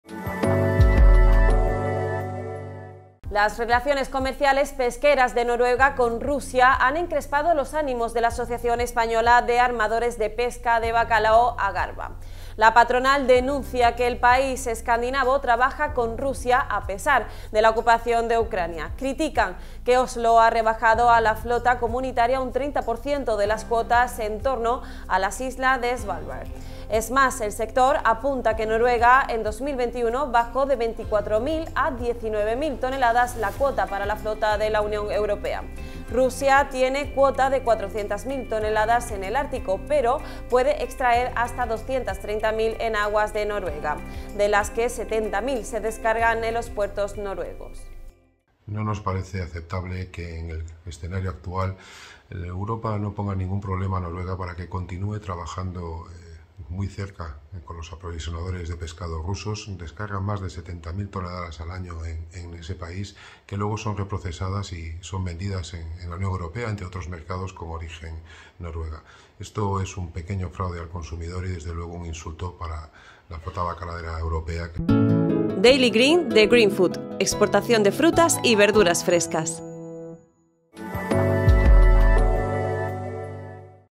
Crispación en la flota española ante la colaboración de Noruega con Rusia.
Las relaciones comerciales pesqueras de Noruega con Rusia han encrespado los ánimos de la Asociación española de Armadores de Pesca de Bacalao (AGARBA). La patronal denuncia que el país escandinavo trabaja con Rusia a pesar de la ocupación de Ucrania. Es más, critican que Oslo ha rebajado a la flota comunitaria un 30% las cuotas en torno a las islas de Svaldbard. Es más, el sector apunta que Noruega, en 2021, bajó de 24.000 a 19.000 toneladas la cuota para la flota de la Unión Europea. Rusia tiene cuota de 400.000 toneladas en el Ártico, pero puede extraer hasta 230.000 en aguas de Noruega, de las que 70.000 se descargan en puertos noruegos.